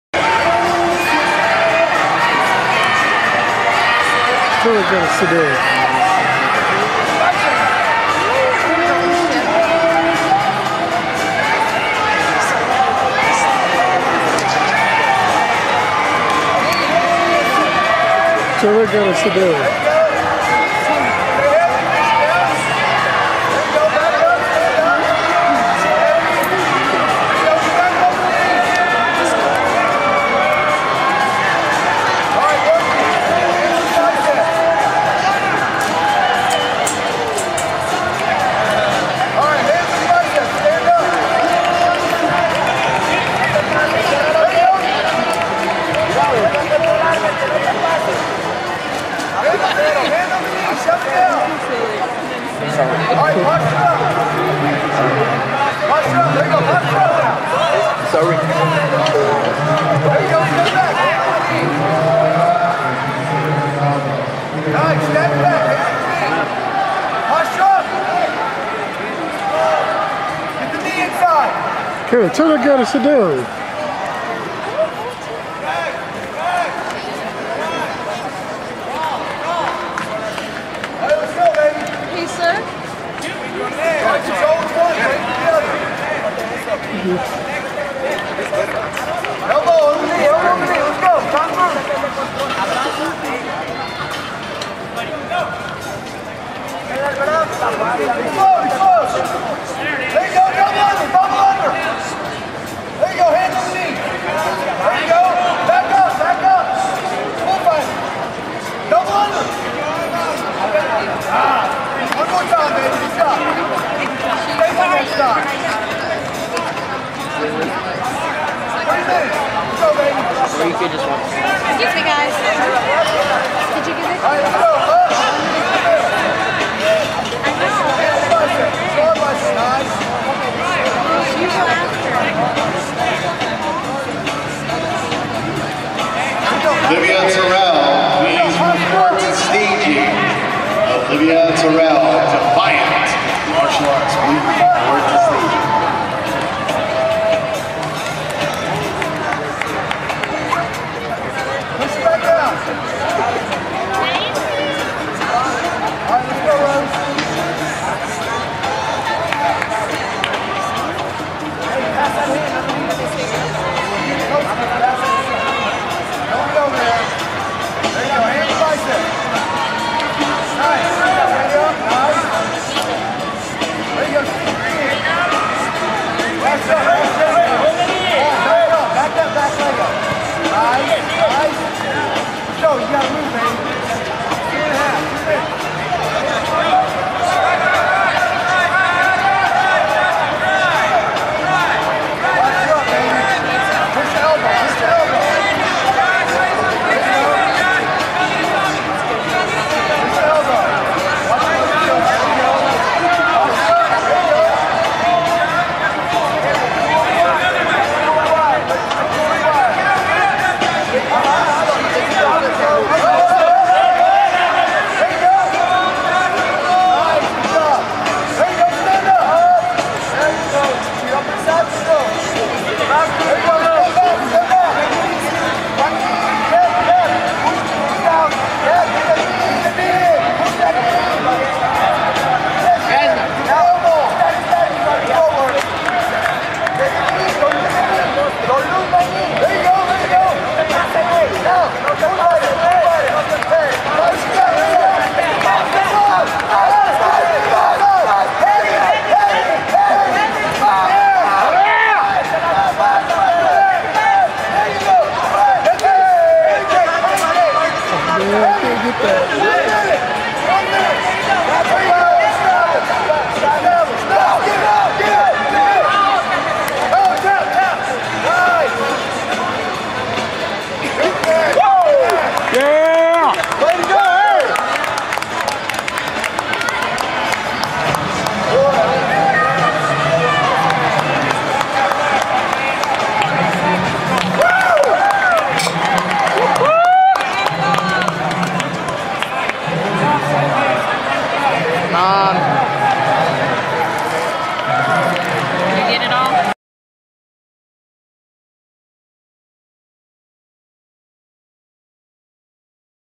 Team Rivas BJJ - Ariel Palmer - Five Grappling Texas 2
Team Rivas Brazilian Jiu- Jitsu out of Conroe, TX. Ariel Palmer on her finals match in the blue belt division.